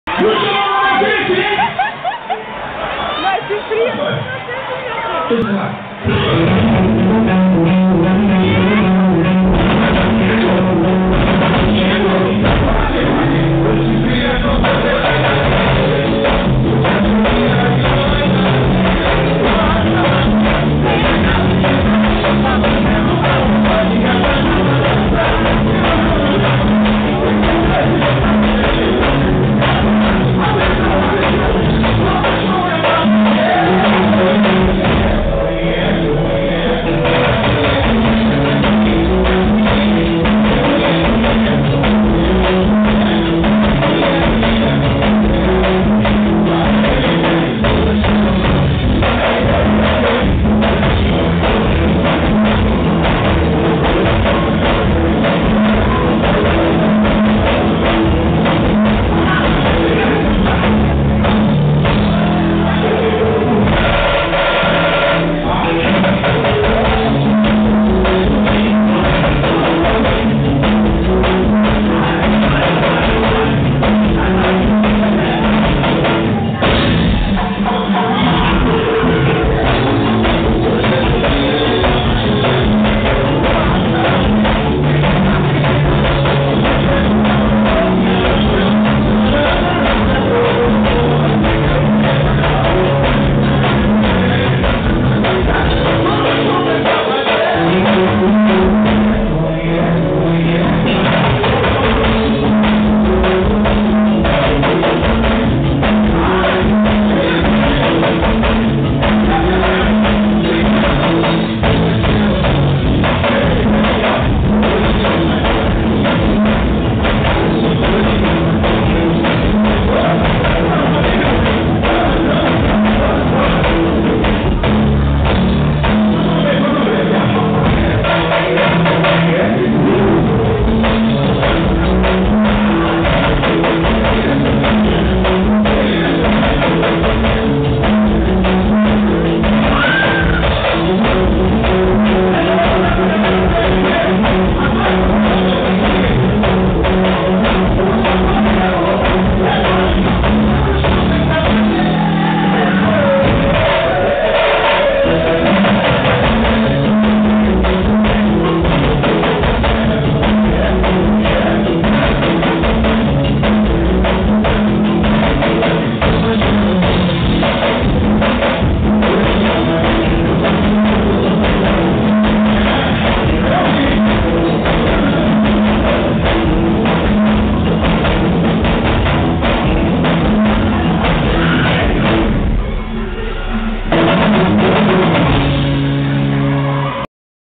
INHECO - RAGHATONI NO MASSIFOLIA
APRESENTAÇÃO DA BANDA RAGHATONI NO MASSIFOLIA, 10 MIL PESSOAS PRESENTES NO MAIOR EVENTO DE PAGODE DO INTERIOR DA BAHIA. VITÓRIA DA CONQUISTA-BAHIA-BRASIL.
BLOCO MASSICAS